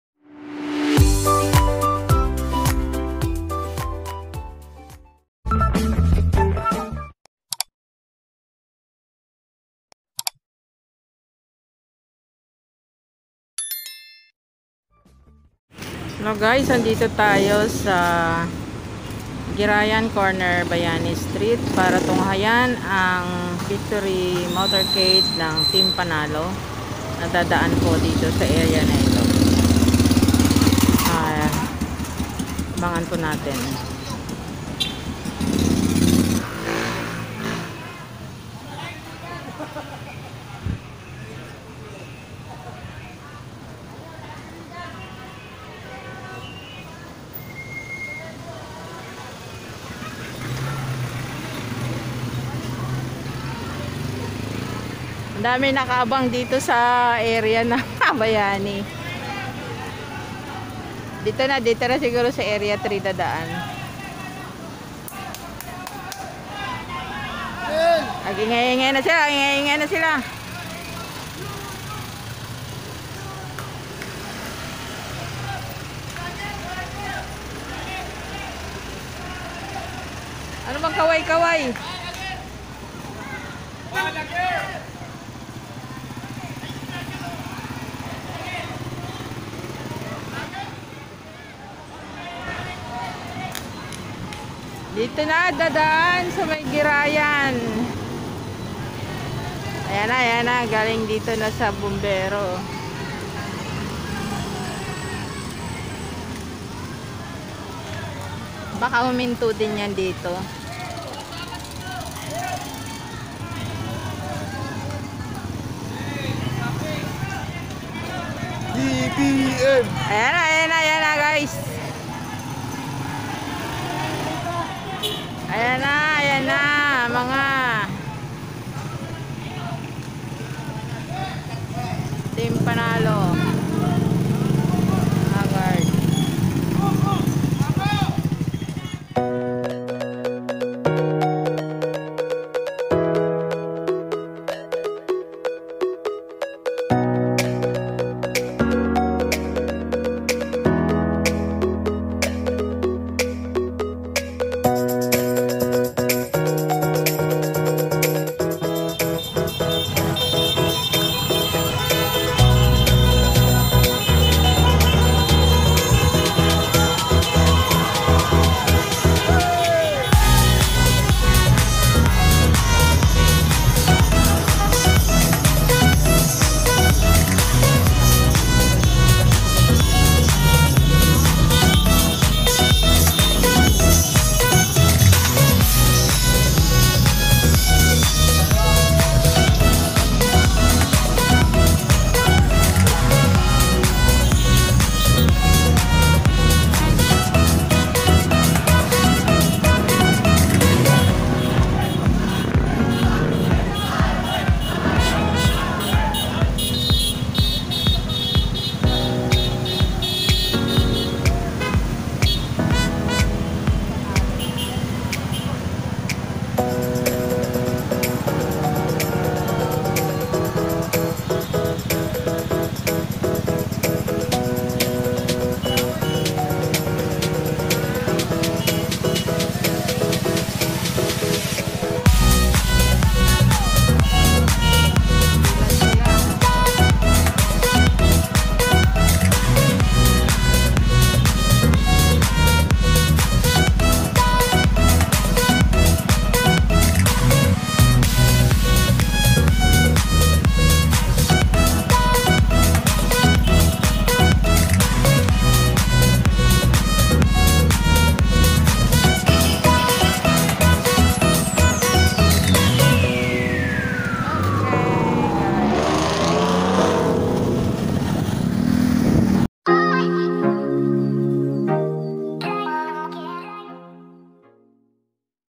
MOTORCADE TEAM PANALO
Hello Guys This is the motorcade of team panalo around quezon city area in district 4 after the team won the recent election 2022. This their way of saying thank you for all who vote them. Hope you watch this vlog  and learn from it. God bless us.